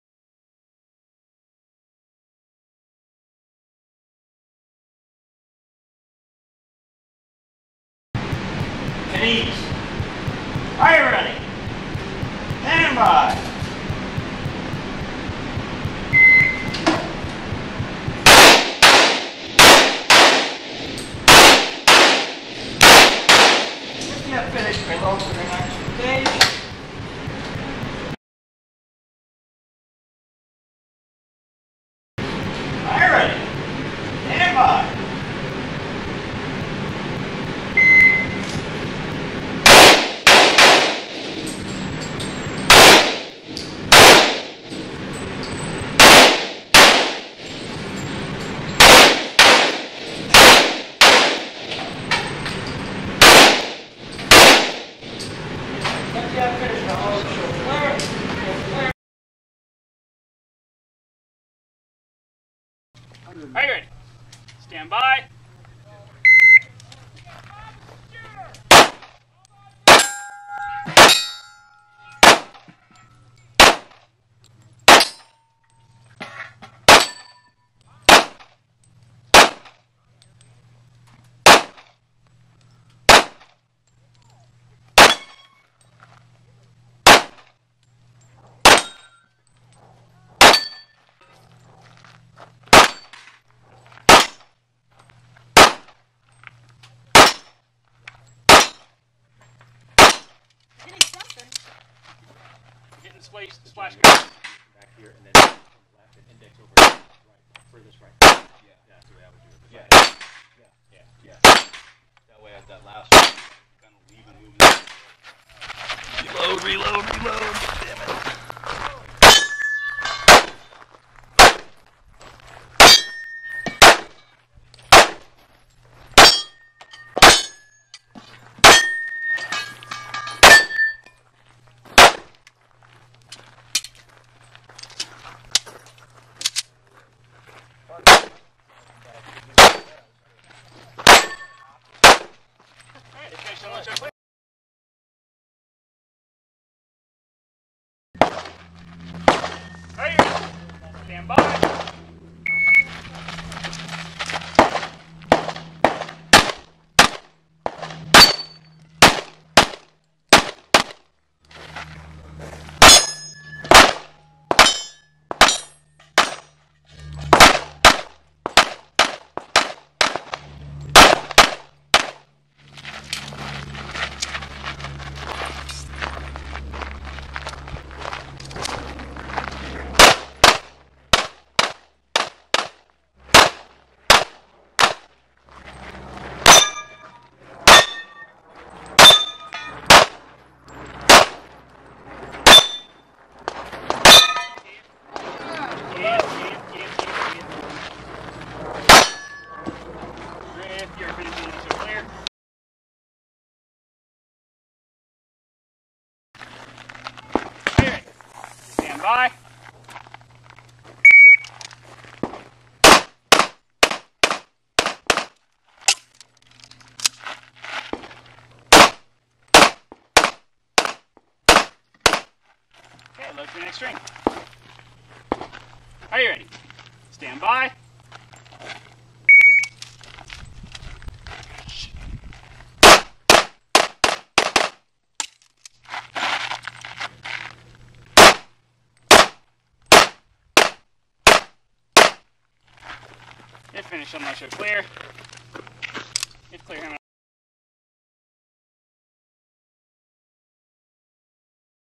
2011-07-31 - Boulder Rifle Club - Lauren
Lauren shooting a USPSA match at the boulder rifle club